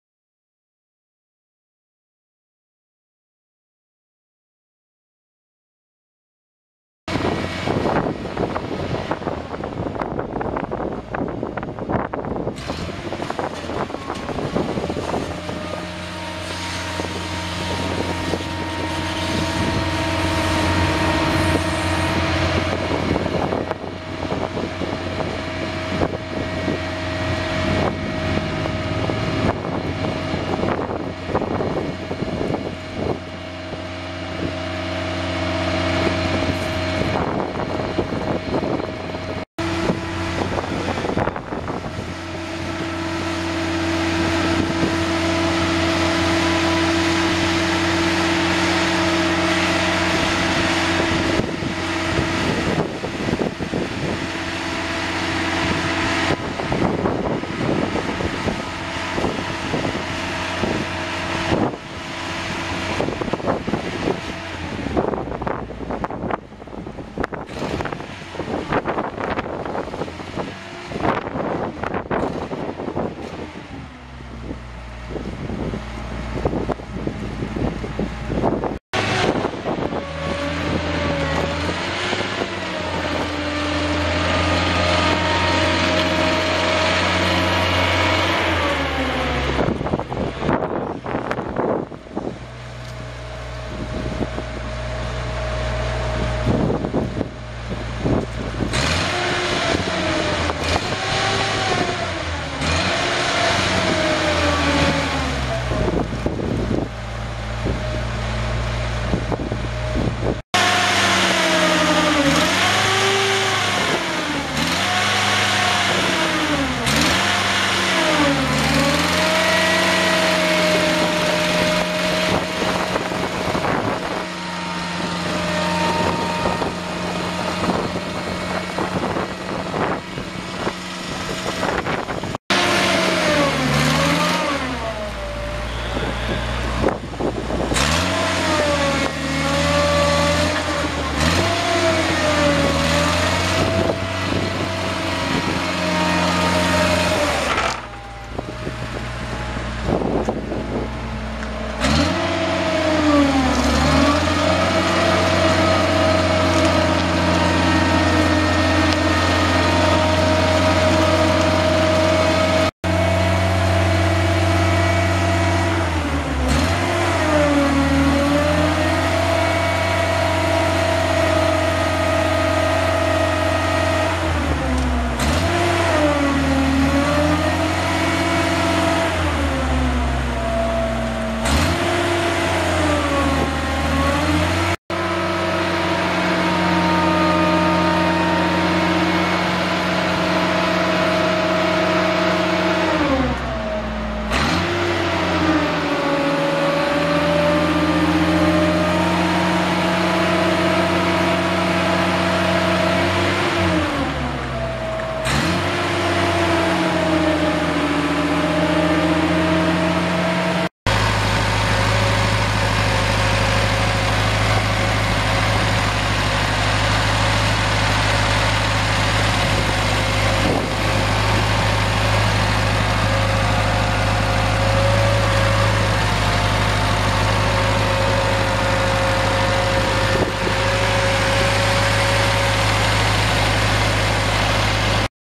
1993 Grove AMZ-66 rough terrain boom lift for sale | sold at auction December 17, 2013
SOLD! North Central Ag and Construction Equipment Auction

1993 Grove AMZ-66 rough terrain boom lift
4,894 hours on meter
Nissan four cylinder dual fuel engine
Gas or propane
Hydrostatic transmission
Two wheel drive
Crab steer
Circle steer
66&#039; working height
500 lbs platform capacity
Basket rotate and tilt
Turret rotate
60"W x 35"D platform
43&#039;H rails
Platform and ground controls
Ground and platform outlets
110V platform outlet
Foot control safety switch
Platform tool tray and weather cover
Turret transport lock pin
Battery main disconnect switch
Chassis tie downs
Safety decals
Ground and platform auxiliary power controls
15-19.5NHS pneumatic tires
Hydraulic tank model GH274829
Fuel tank model 002345
Recently replaced fuel pump
Cosmetic damage
Serial 27647
Unit# 4353